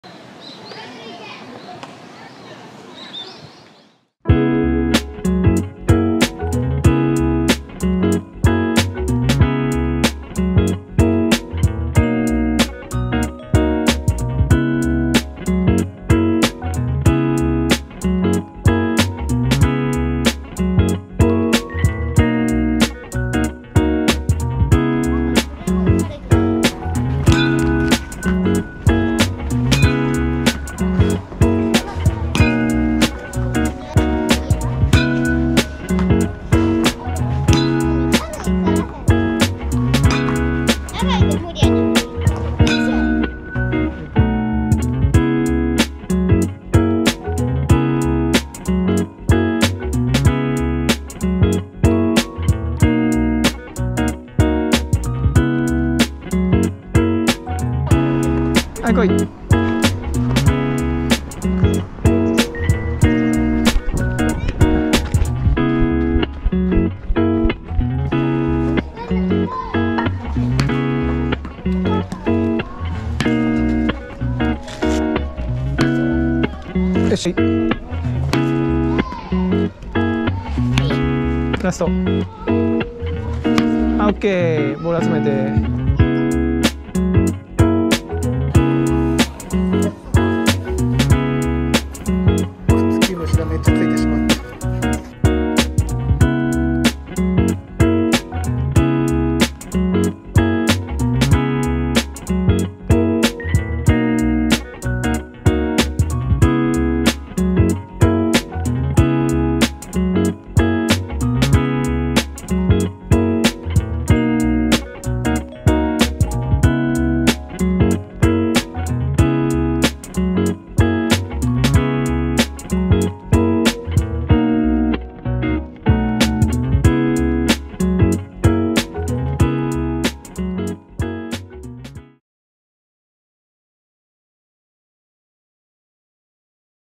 Weekend baseball （2022/10/08-10）
3連休も野球中心でした。
が、中日と最終日は雨模様で少し残念。最終日は練習試合がありましたが、雨天で2回裏で中断という形に（それでも1時間くらいは試合をしていたんですが...）